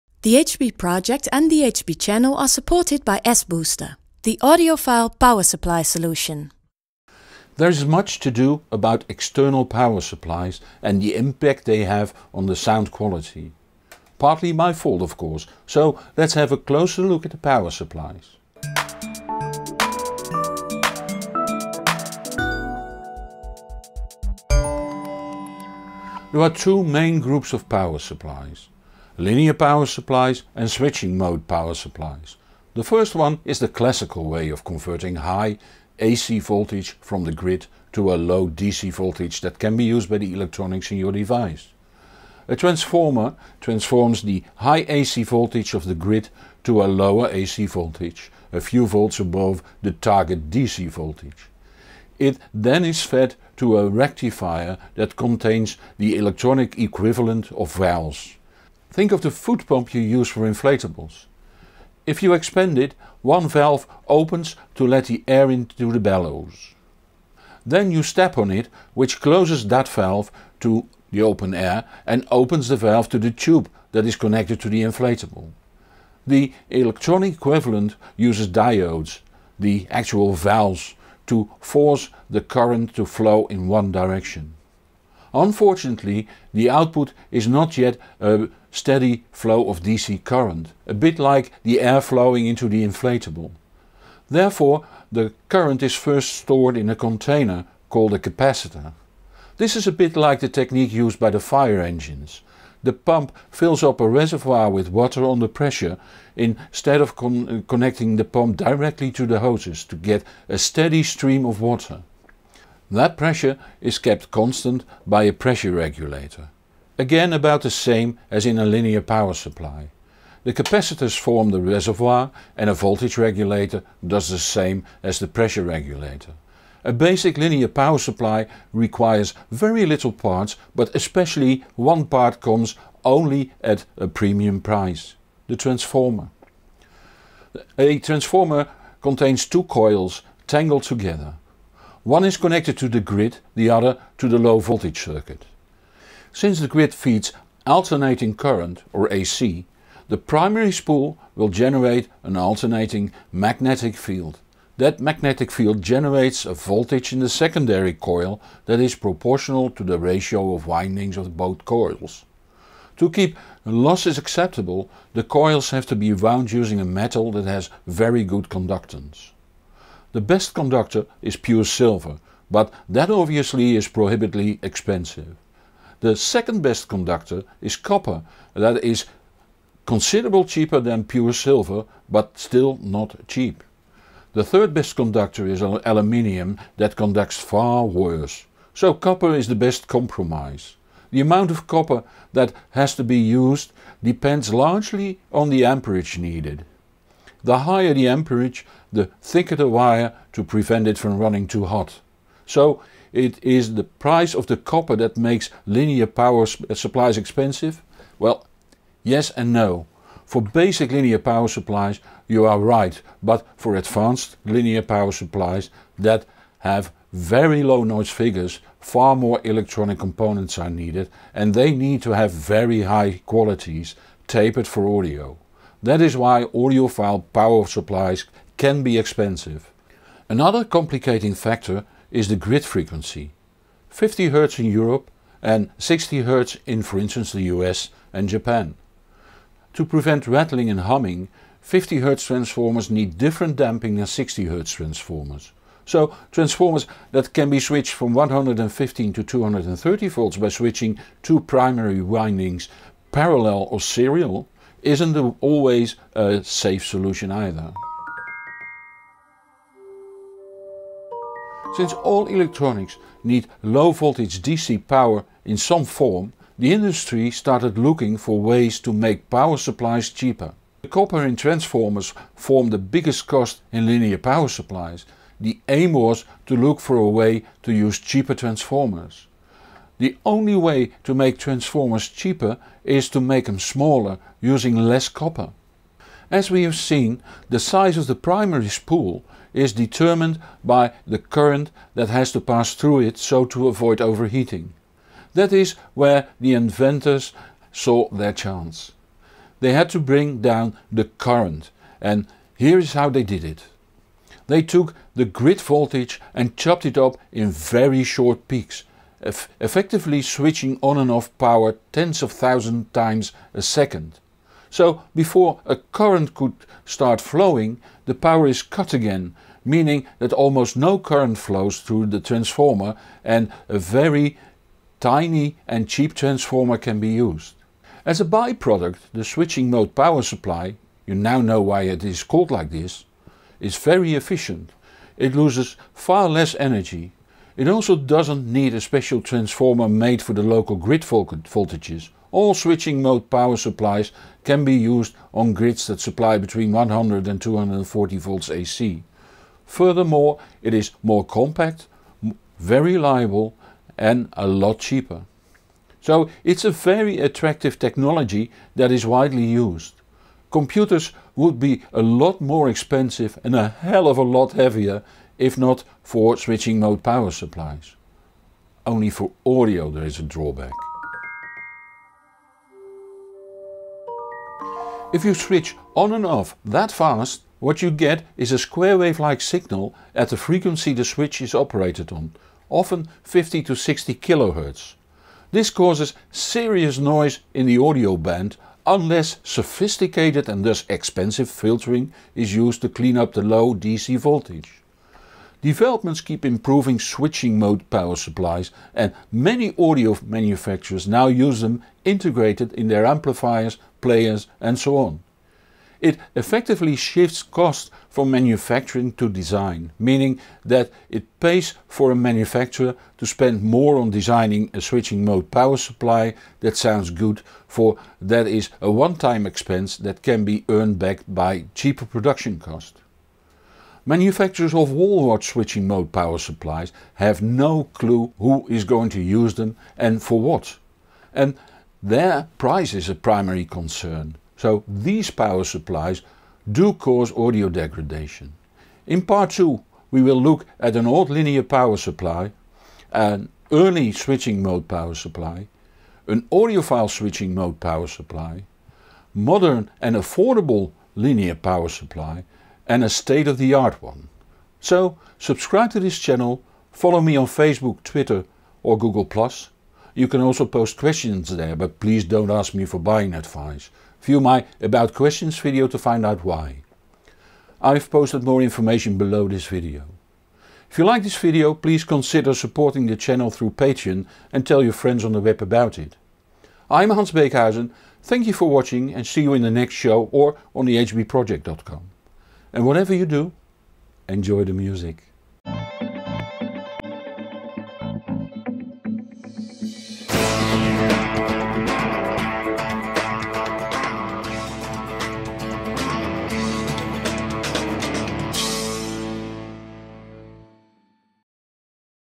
External power supplies part 1 introduction
There is much to do about external power supplies and the impact they have on the sound quality. Partly my fault, of course. So let’s have a closer look at power supplies. (English and Dutch subtitles - Nederlands ondertiteld)